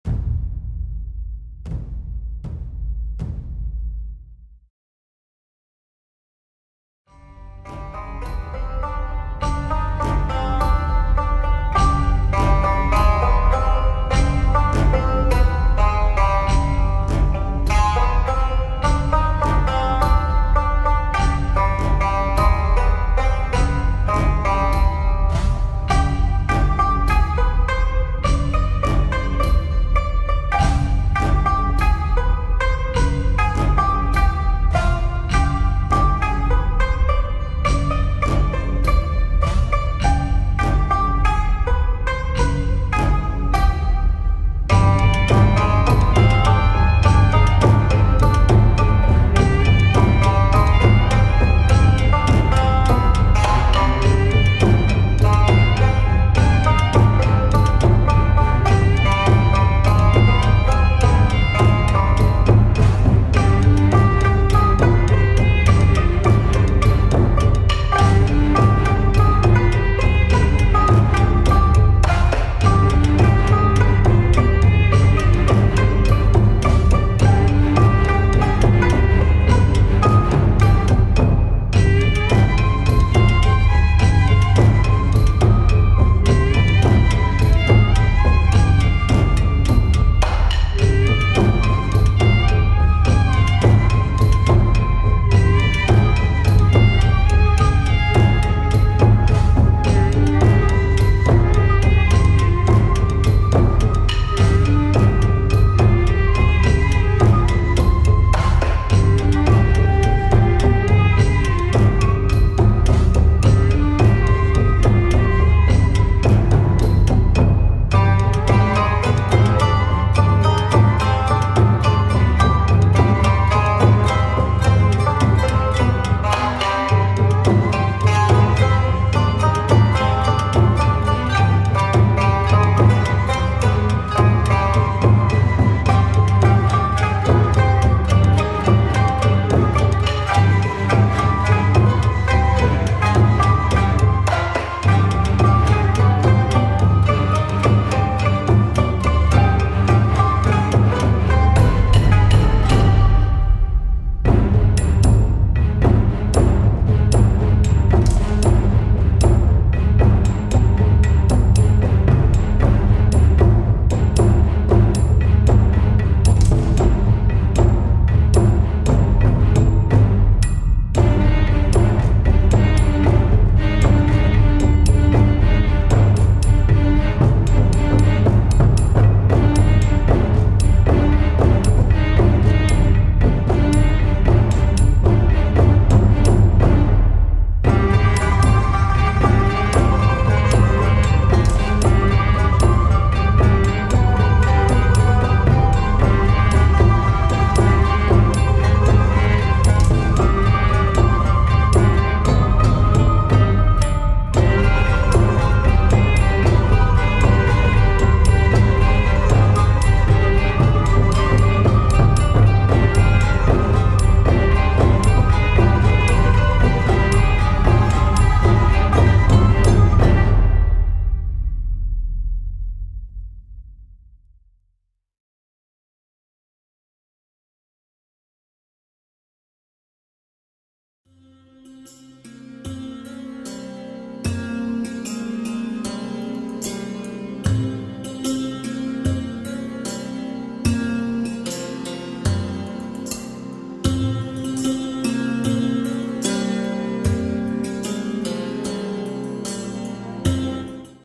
Digital Speedpaint - KHAJIIT (The Elder Scrolls Fan-Art) - Jimmy Nijs Art
One of the results of the raffle I held earlier this month. Congratulations to @johnwuux on Twitter for winning this awesome portrait of his Elder Scroll Online character!

Wanna help support me further? Why not purchase one of my spooky designs on Teespring!